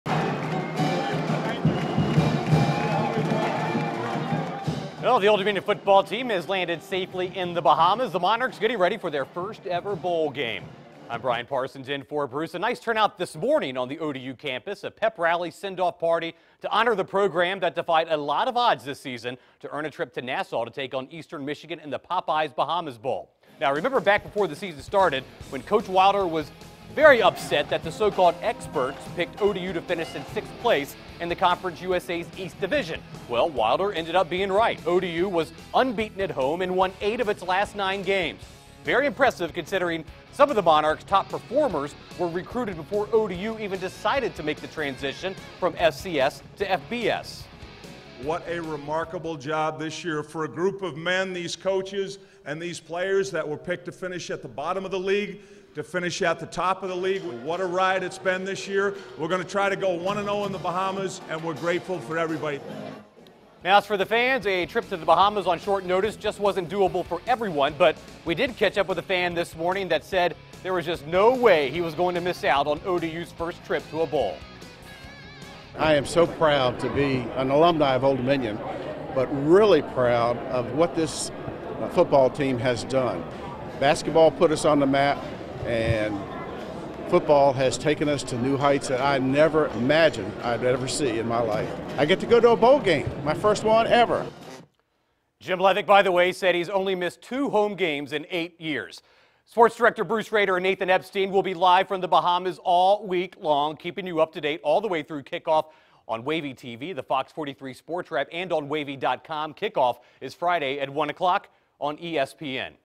Old Dominion Departs For The Bahamas Bowl
The Old Dominion Football Bowl left Norfolk this morning for their Bahamas Bowl match-up against Eastern Michigan.